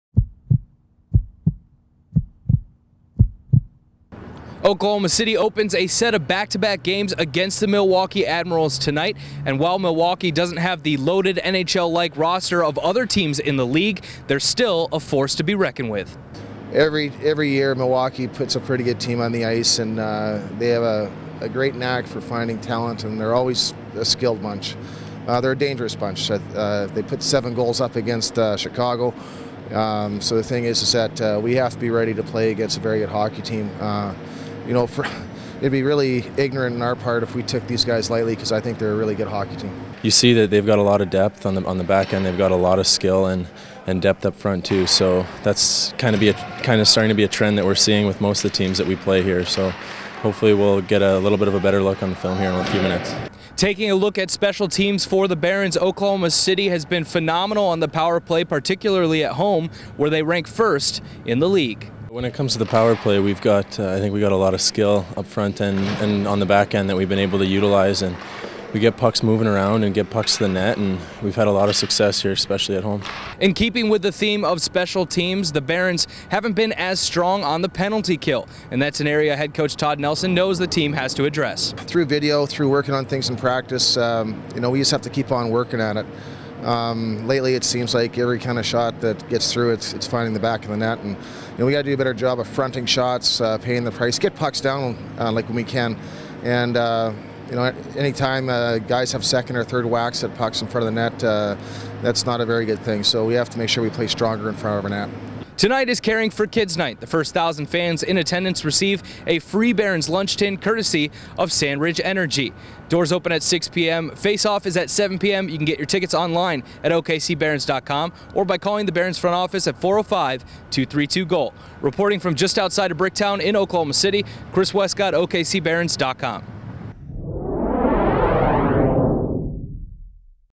BARONS BEAT: OKC Opens Milwaukee Series, Focuses on PK
OKCBarons.com's Chris Wescott reports on the team as they host Milwaukee tonight.